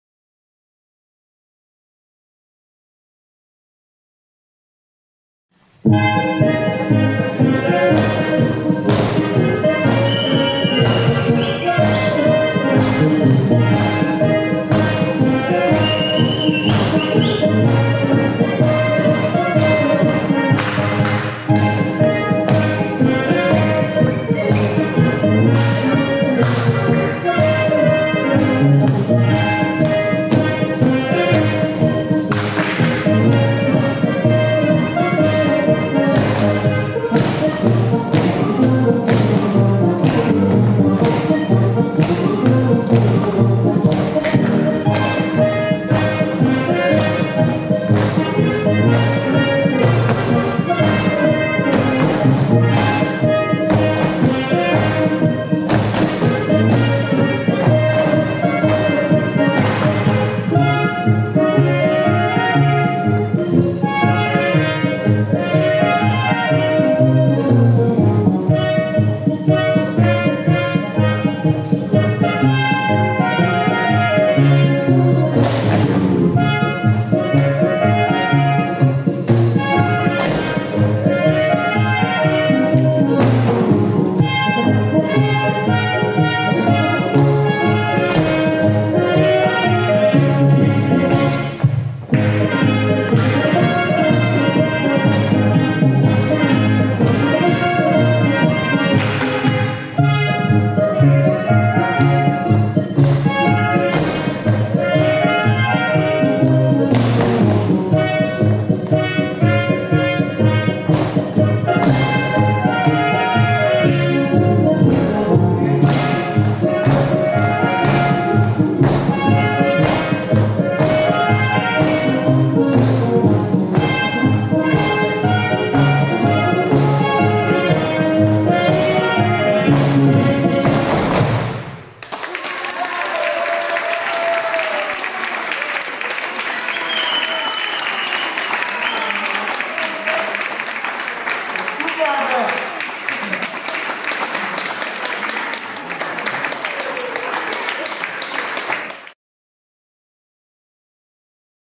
2024 04 19 TAG des TAZES HIDIGUT ALTDORFER TANZGRUPPE "GLÜKLICHES WIEDERSEHEN"
Am  TAG des TANZES  wurde die  Choreographie "GLÜCKLICHES WIEDERSEHEN"  durch die ungarndeutsche HIDIGUT ALTDORFER TANZGRUPPE  unter der Leitung von Elisabeth BOBOK aufgeführt, Choreograph Z. WENCZL